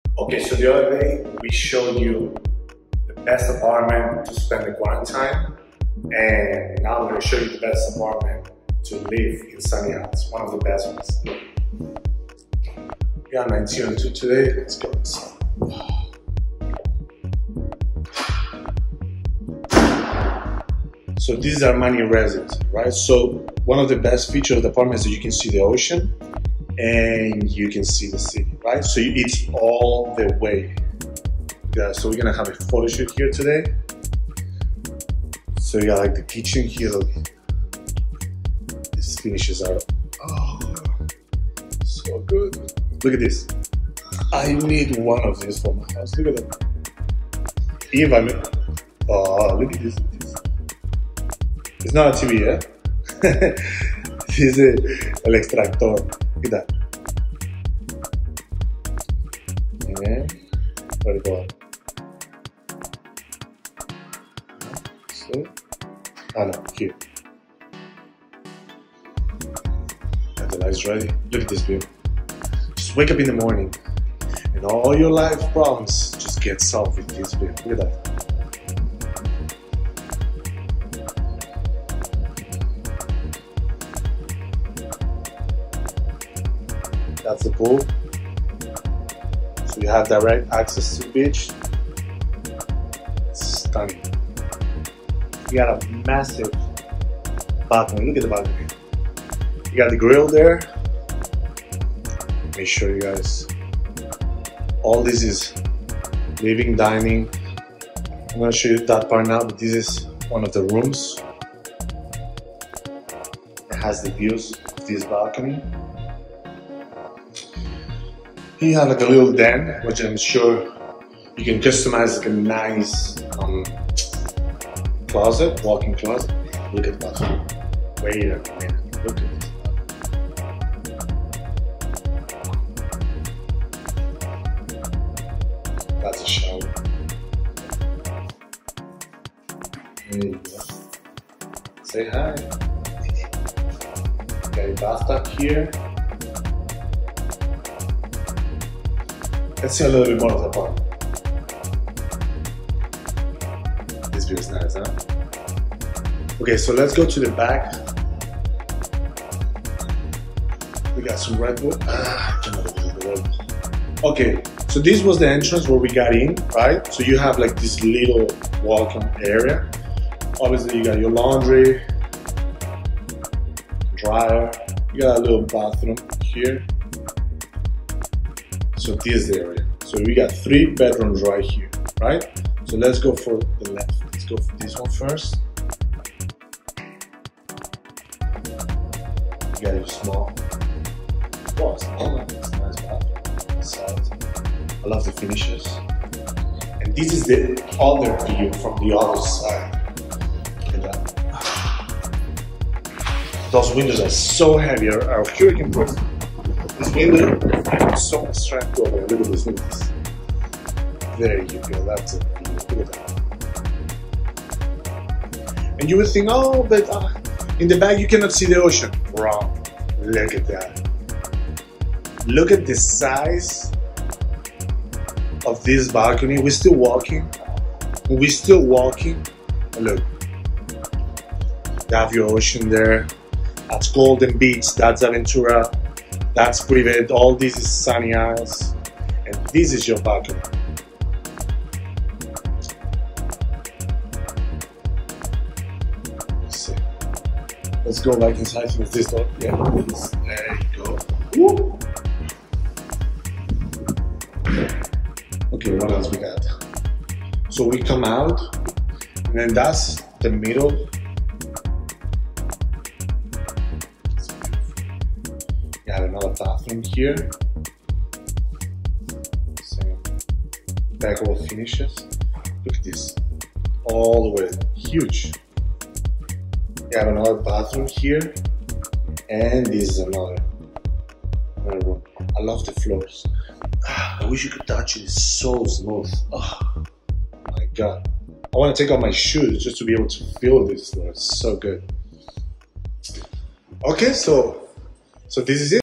Tour of Armani Residences #1902 Unit | by Dezer Development & Armani
Luxurious oceanfront unit at freshly minted Residences by Armani Casa Unit B 1902. Residence is completely finished with Porcelain White Carrara flooring (24 by 48)throughout the interior and exterior. ⁣ready to move in ⁣
The unit boasts stunnning ocean and intracoastal views compliments of its flow-thru layout. This residence has 4 bedrooms, den and service area and 5.5 bathrooms, private high-speed elevator access to foyer, 10-foot ceilings, floor to ceiling windows and 2 expansive balconies overlooking the intracoastal and ocean. ⁣
Asking $3,590,000.